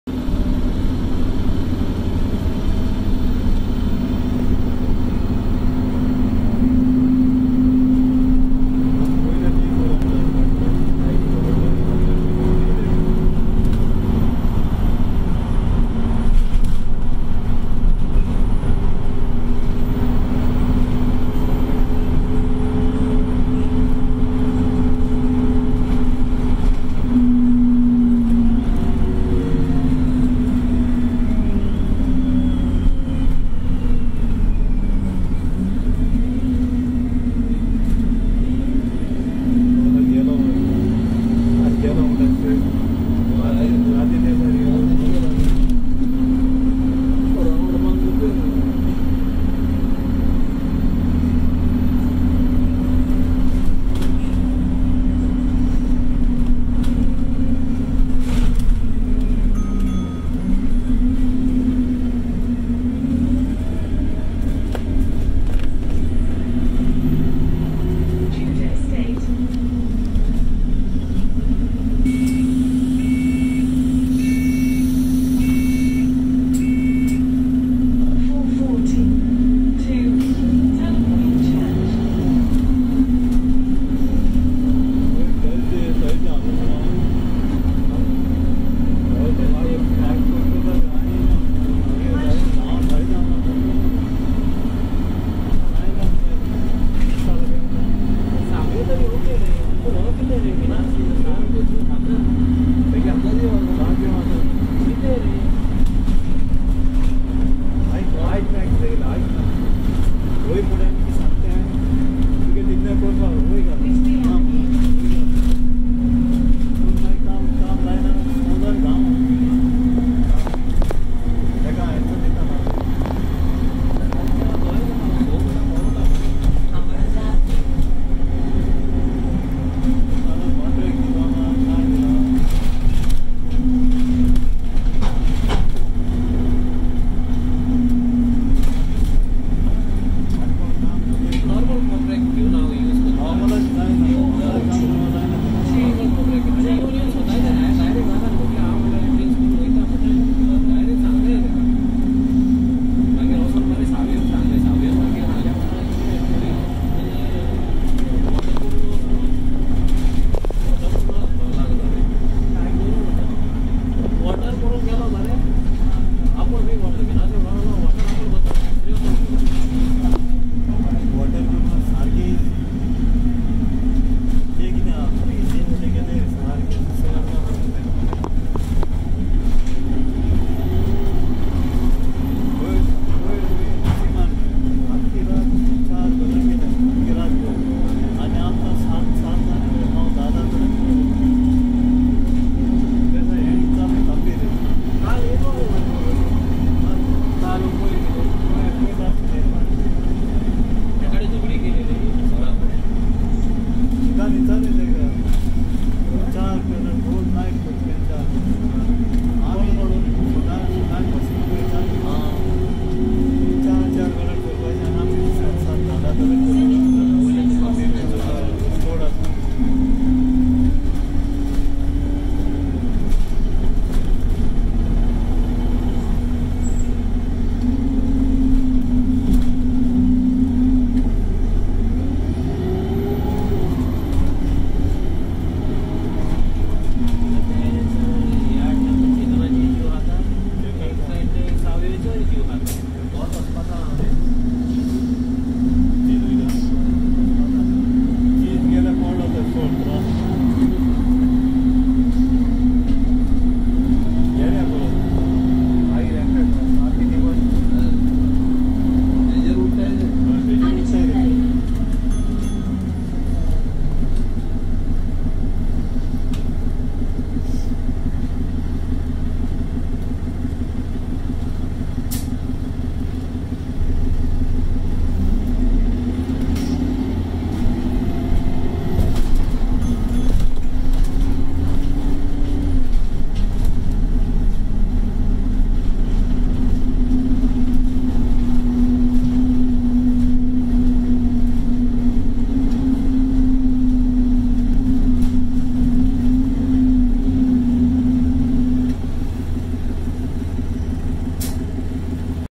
RATP London DME30362 YX19 OMV
Here we see a journey on RATP London Enviro 200 MMC DME30362 YX19 OMV seen on route 440 to Wembley.

Thanks for my friend for giving me permission to post.